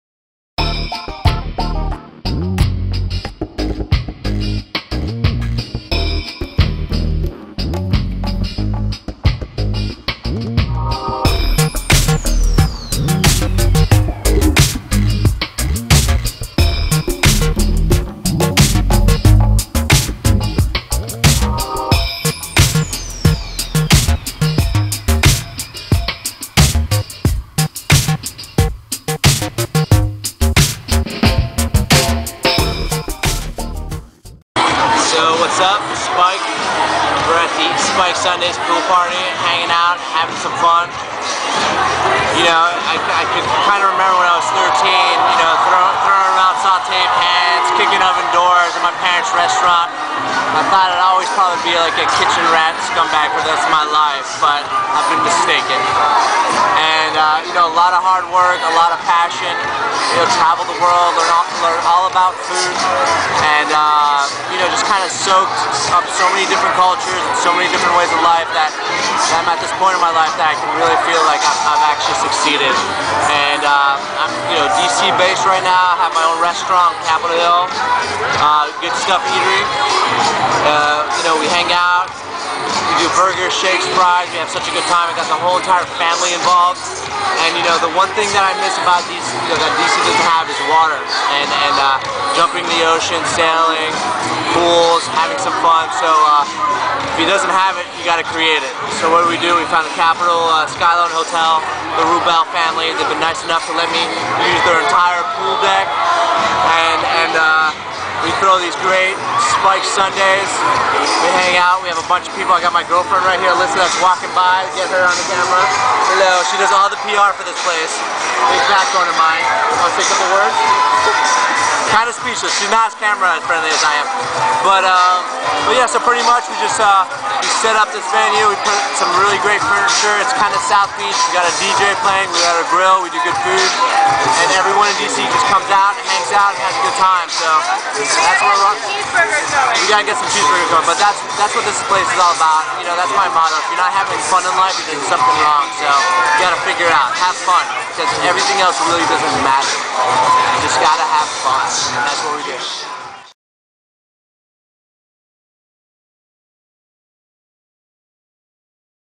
Pool Party: Spike'd Sundays with Spike Mendelsohn
Spike'd Sundays with Spike Mendelsohn

Former Top Chef contestant discusses his restaurant and Sunday pool parties in Washington, DC.